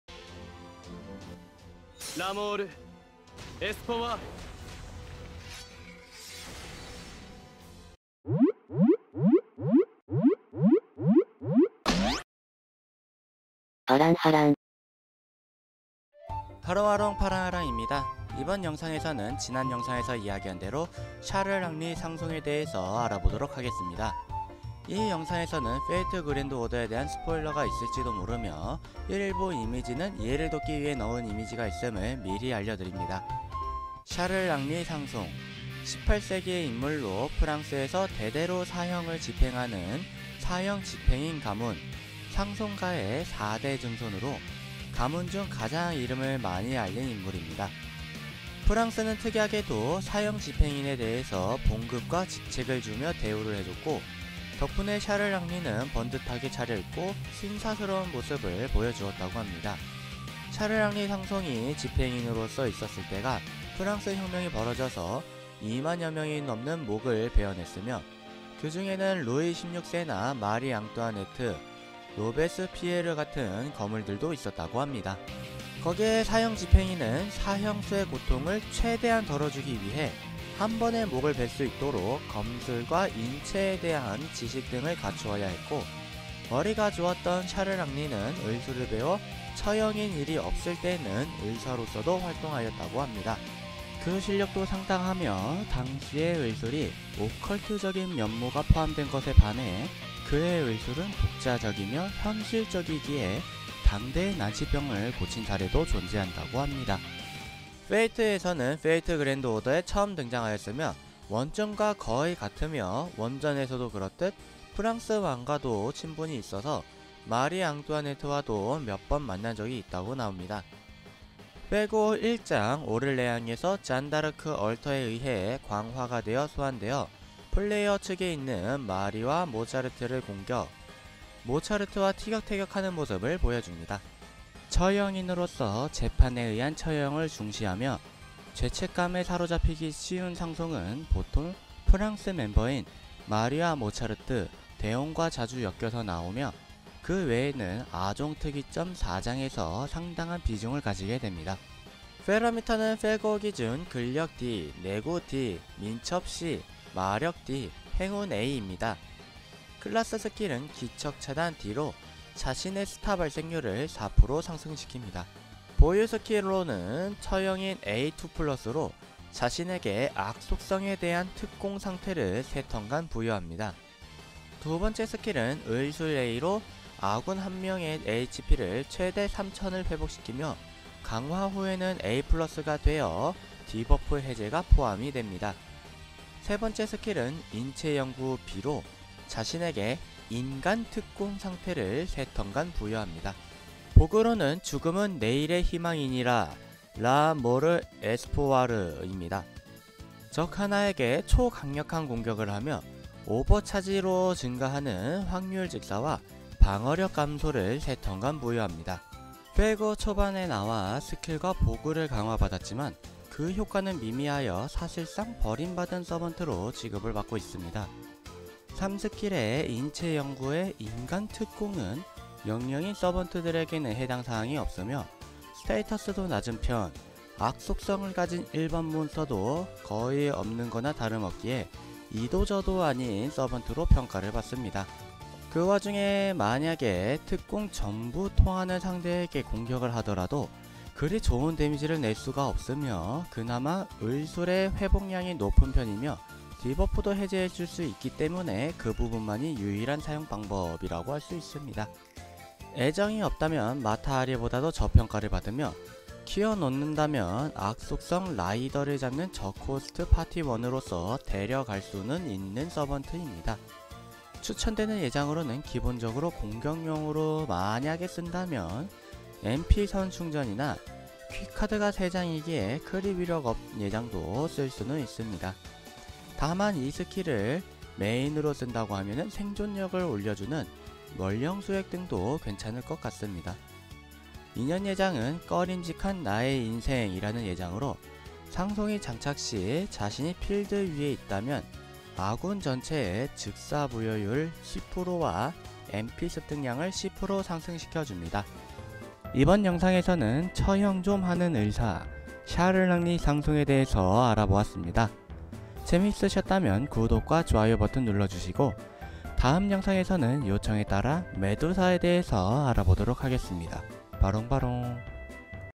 [fate/GO]샤를 앙리 상송에 대해서
쓸때없는 지식1: 상송의 무기는 엑시큐셔너 소드라는 참수를 목적으로 만들어진 무거운 검입니다.

 2: 상송은 사형 폐지법에 열렬한 지지자 였으며, 탄원서를 몇번이고 제출했다고 합니다.

 3: 기요틴(단두대)의 발명가는 외과의사인 앙투안 루이(18세기)이며, 그 외의 6명의 기술자들의 조언과 도면으로 만들어졌다. 최종적으로 루이 16세가 칼날의 디자인을 감수했고, 기요틴 발명에 기여한 인물 중 유일하게 처형당한 인물이 루이 16세입니다.